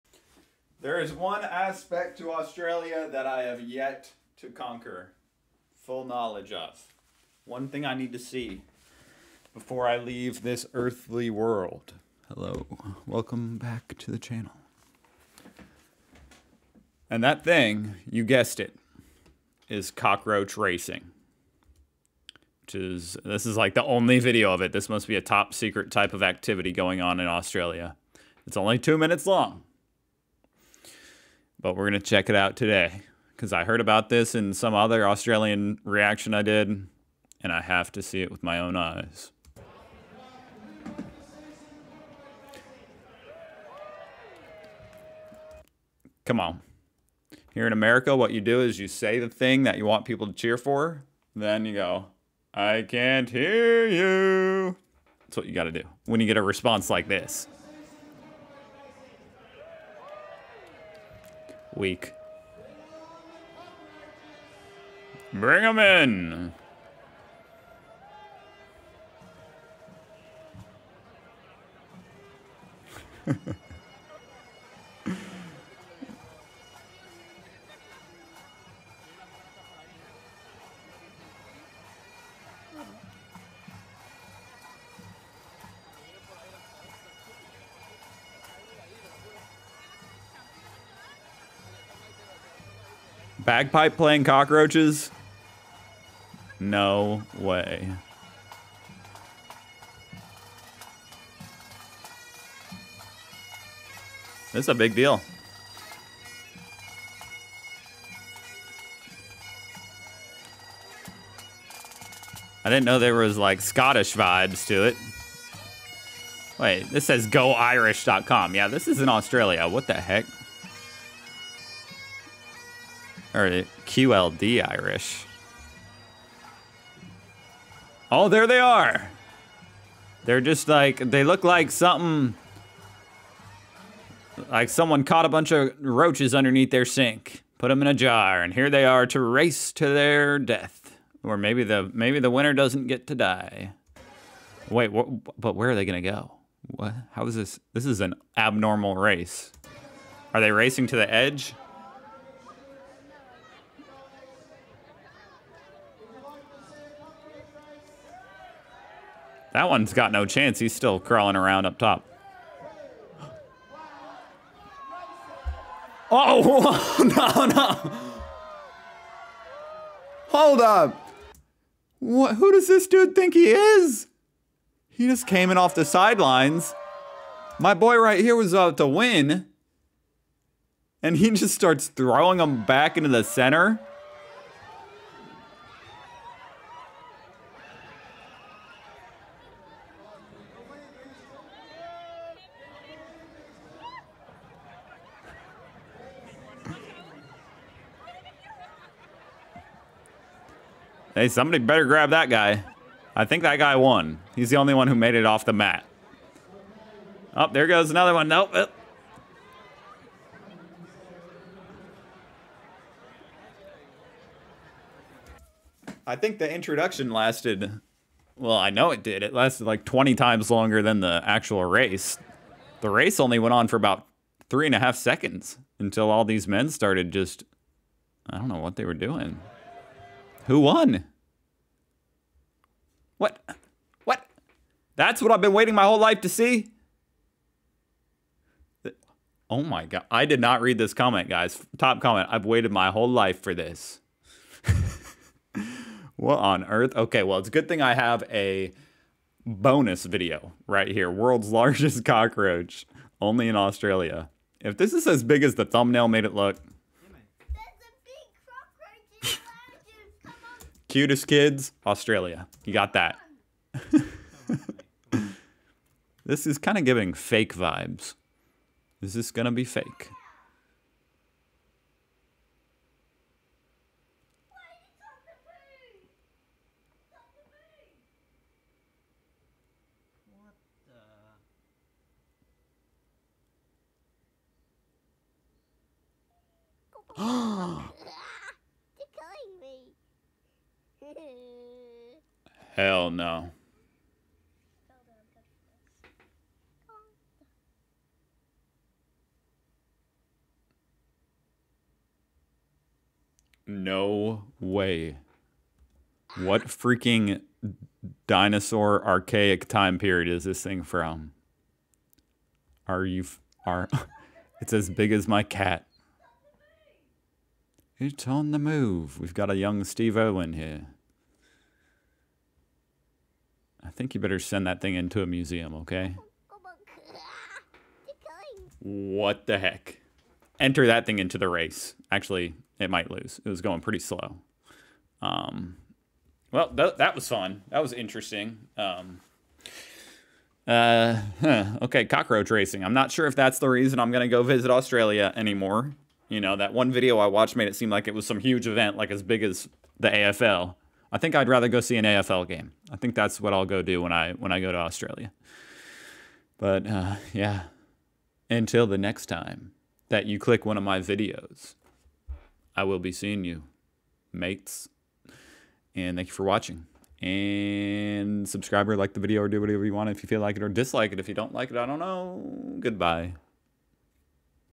American Reacts to Australian Cockroach Racing
Wow, what a sport. Who won!? Straight out of Australia. Have a great arvo.